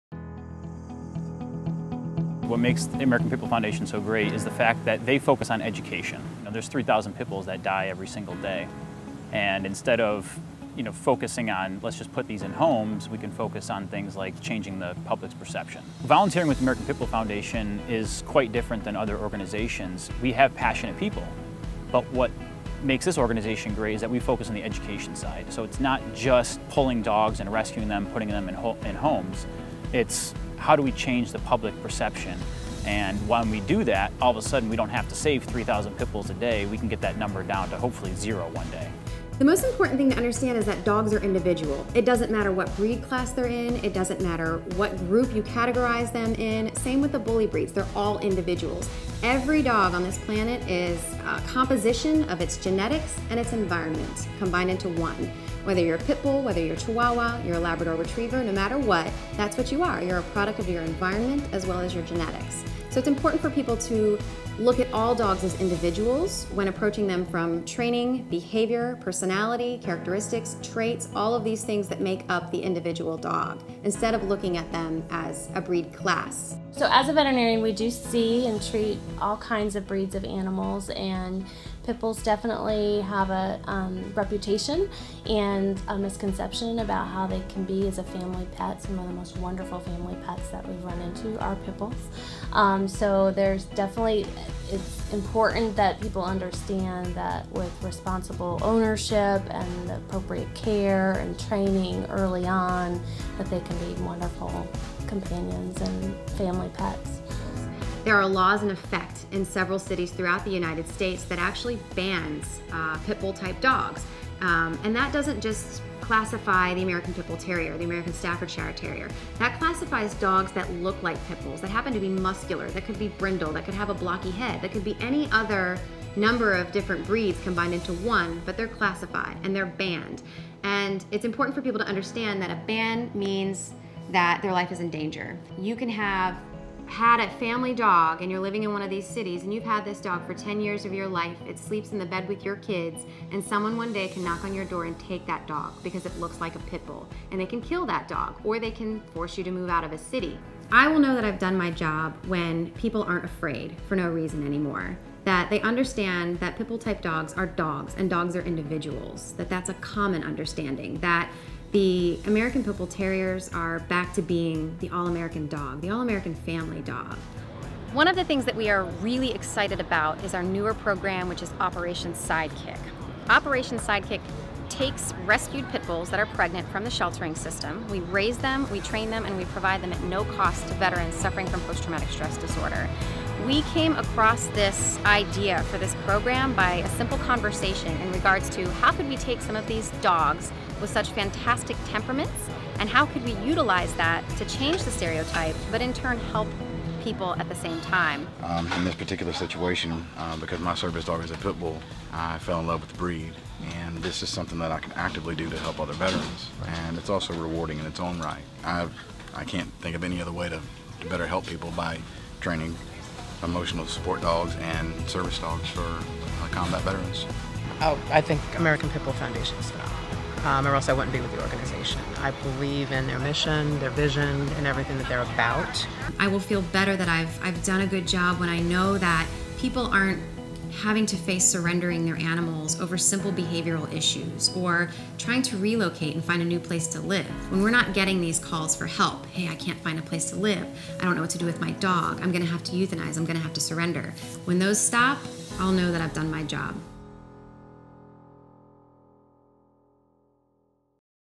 Inside the American Pit Bull Foundation
APBF Founder, Sara Enos Ondrako and APBF volunteers talk about the importance driving the purpose behind the mission of the organization. From Humane Education to decreasing unnecessary euthanasia and suicide among veterans, the American Pit Bull Foundation tackles community animal welfare issues while promoting responsible dog ownership.